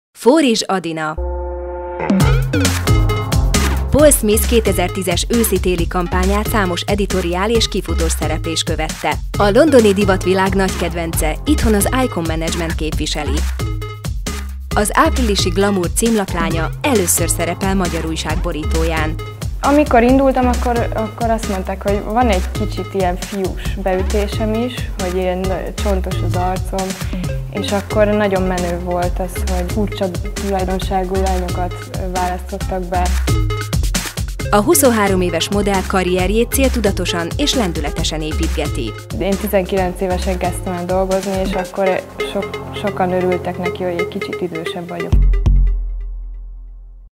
Fórizs Adina
Adina a GLAMOUR Women of the Year 2011 jelöltje, legjobb modell kategóriában.